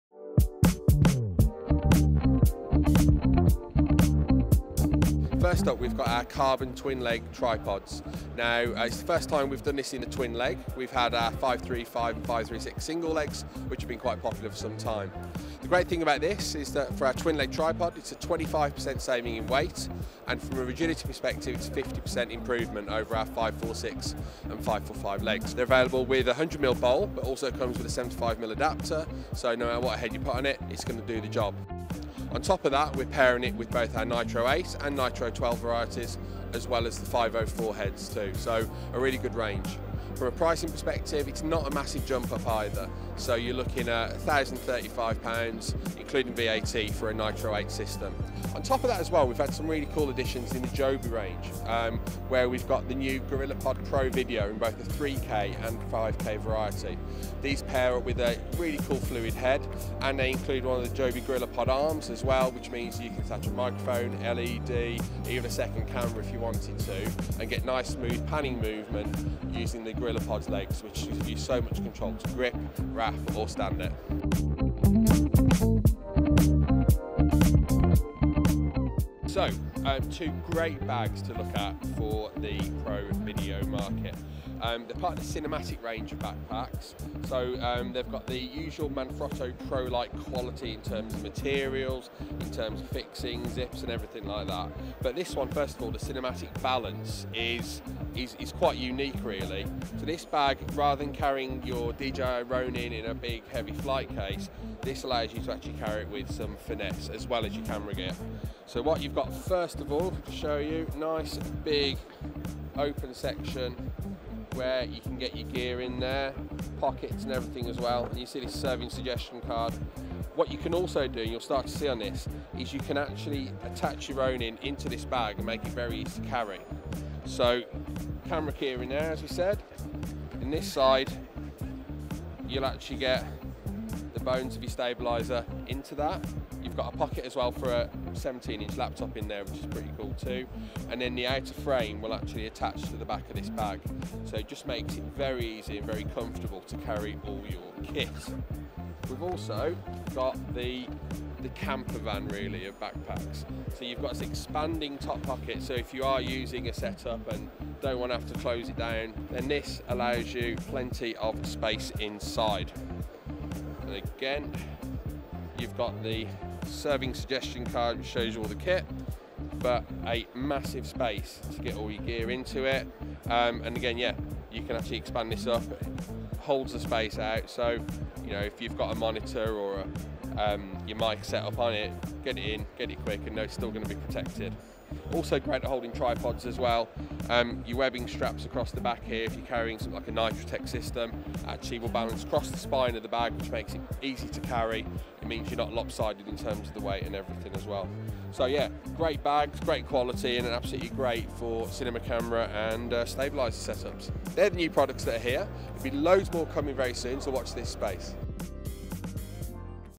Manfrotto & Joby at IBC 2018
We catch up with the latest in products and solutions from Manfrotto & Joby on-stand at IBC 2018!